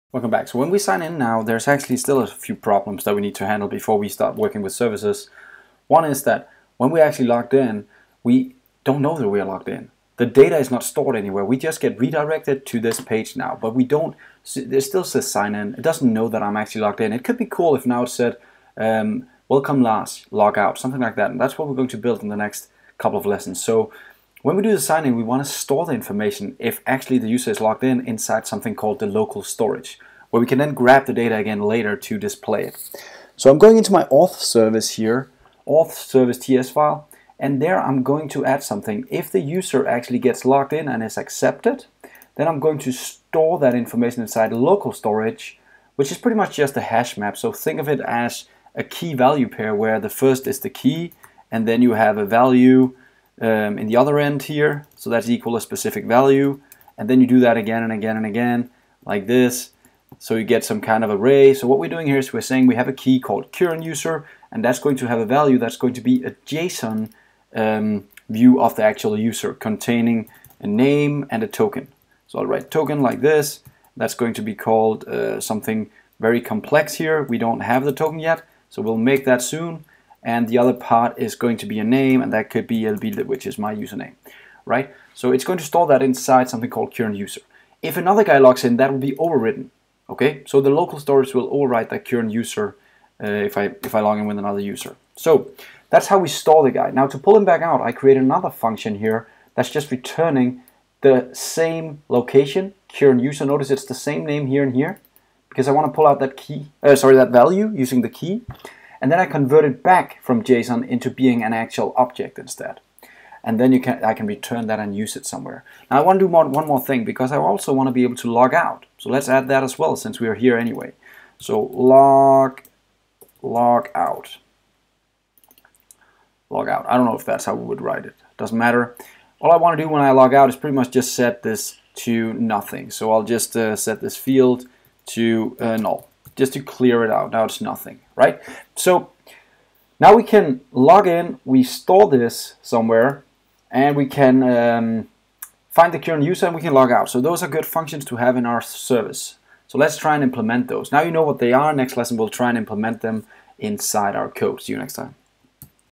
using localstorage to store login information
This Series is about building a full-stack Web Applications using MEAN stack with Angular2 as the A in MEAN
We will build a course-planner application and will address most of the issues when developing a real online web application.

If you want more information of the MEN of MEAN and how we build a REST API in Mongo, ExpressJS and NodeJs go my series: